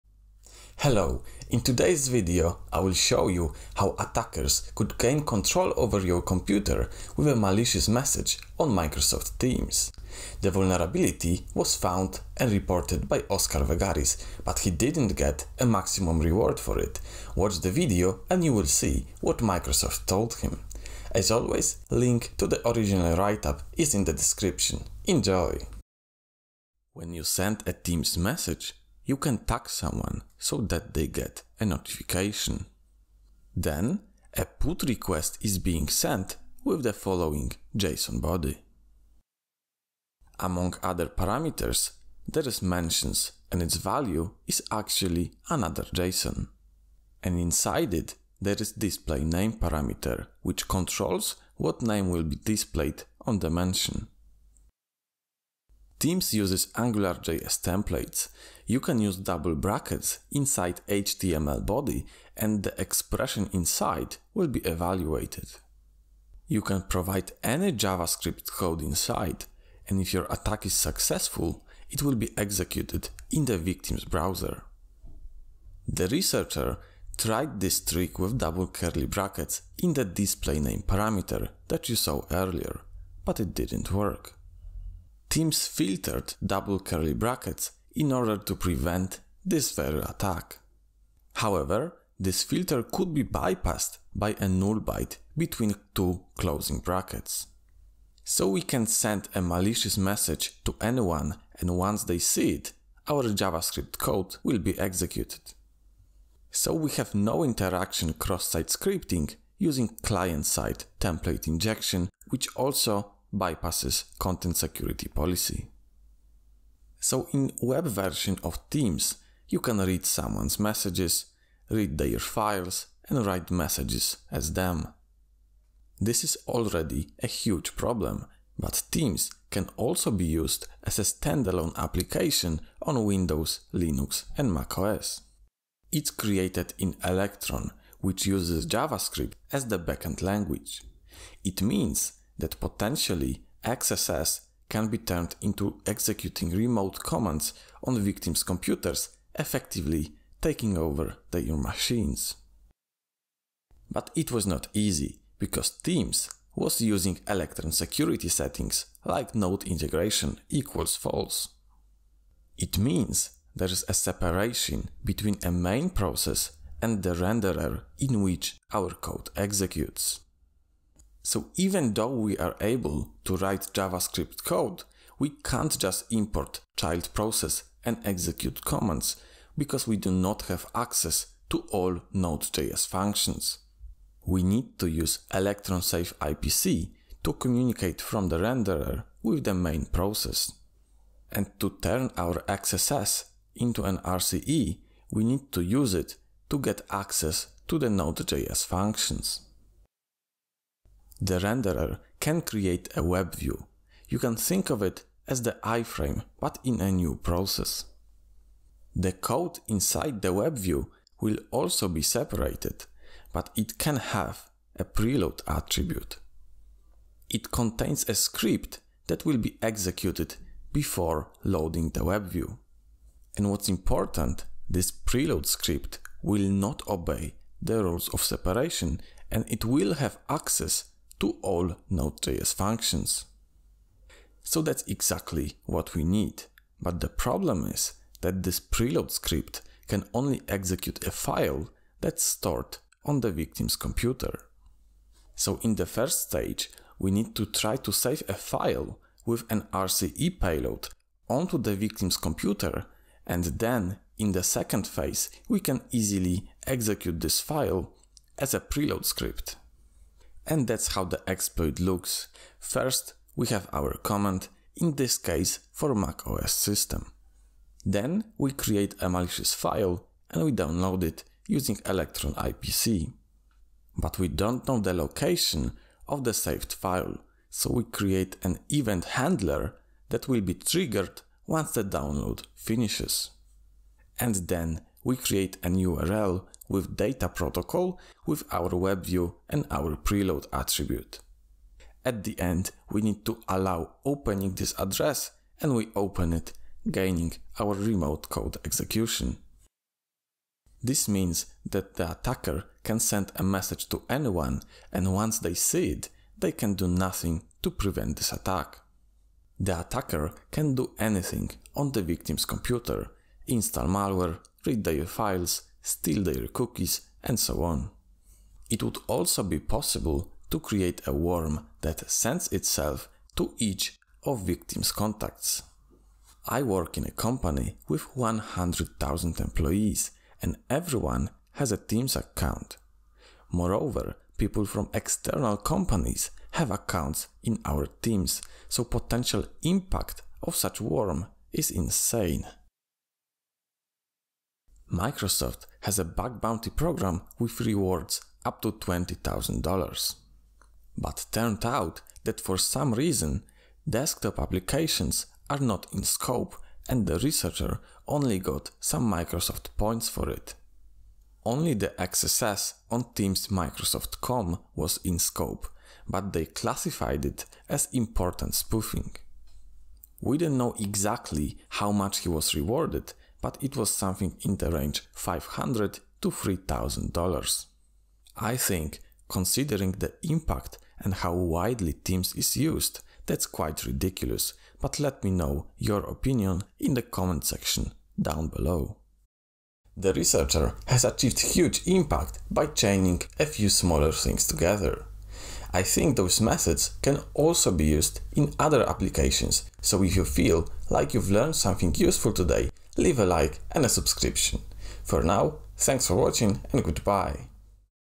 MS Teams - One message that can install malware on your computer - Bug Bounty Reports Explained
This video is about the RCE vulnerability in desktop clients of Microsoft Teams application. In describes multiple techniques like XSS via AngularJS client-side template injection and Electron renderer escape.

The bugs have been fixed since the end of October, 2020.

Timestamps:
00:00 Intro
00:27 Client-side template injection
02:08 Turning the XSS into an RCE